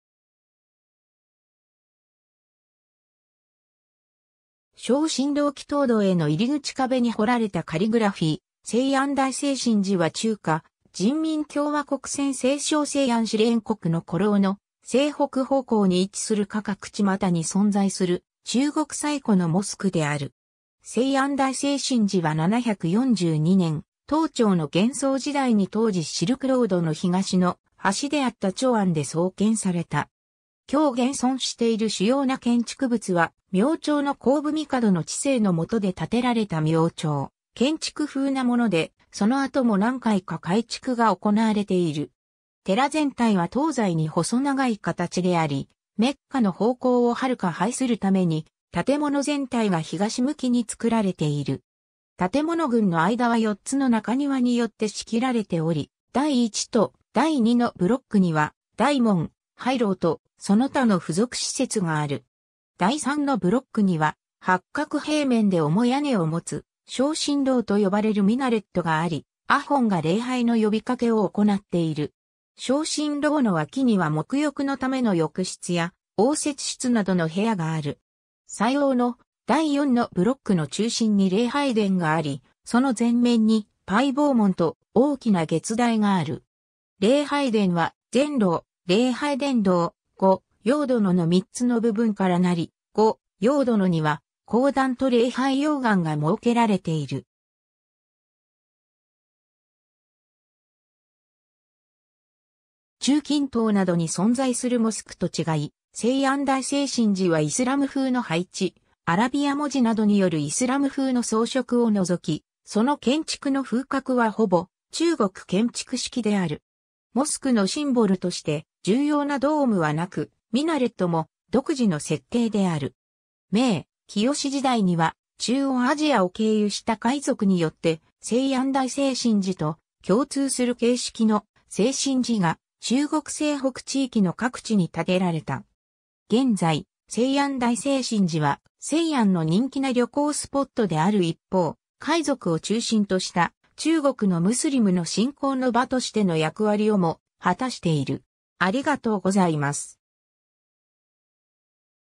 西安大清真寺
省心楼（ミナレット） 祈祷堂への入り口 壁に彫られたカリグラフィー 西安大清真寺（せいあんだいせいしんじ、簡体字中国語: 西安大清真寺、繁体字中国語: 西安大清真寺、拼音: Xī’ān Dà Qīngzhēnsì）は中華人民共和国陝西省西安市蓮湖区の鼓楼の西北方向に位置する化覚巷に存在する中国最古のモスクである。
 西安大清真寺は742年、唐朝の玄宗時代に当時シルクロードの東の端であった長安（今日の西安）で創建された。
 今日現存している主要な建築物は、明朝の洪武帝の治世のもとで建てられた明朝建築風なもので、その後も何回か改築が行われている。
 寺全体は東西に細長い形であり、メッカの方向を遙拝するために建物全体が東向きに作られている。
建物群の間は4つの中庭によって仕切られており、第1と第2のブロックには大門、牌楼と、その他の付属施設がある。
第3のブロックには八角平面で重屋根を持つ「省心楼」（邦克楼ともいう）と呼ばれるミナレットがあり、アホンが礼拝の呼びかけを行っている。
省心楼の脇には沐浴のための浴室や応接室などの部屋がある。
最奥の第4のブロックの中心に礼拝殿があり、その前面に牌坊門と大きな月台がある。
礼拝殿は前廊（ターラール）、礼拝殿堂、後窰殿の3つの部分から成り、後窰殿には講壇（ミンバル）と礼拝窰龕（ミフラーブ）が設けられている。
 中近東などに存在するモスクと違い、西安大清真寺はイスラム風の配置、アラビア文字などによるイスラム風の装飾を除き、その建築の風格はほぼ中国建築式である。
モスクのシンボルとして重要なドームはなく、ミナレットも独自の設計である。
明・清時代には中央アジアを経由した回族によって、西安大清真寺と共通する形式の「清真寺」が中国西北地域の各地に建てられた。
 現在、西安大清真寺は西安の人気な旅行スポットである一方、回族を中心とした中国のムスリムの信仰の場としての役割をも果たしている。